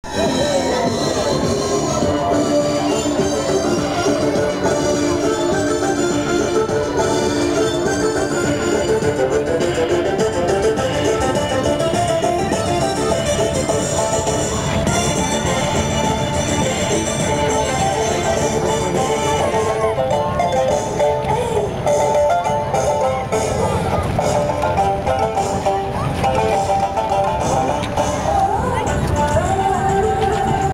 Vancouver 2007 Chinese New Year Parade II
Dancers at Vancouver's 2007 Chinese New Year parade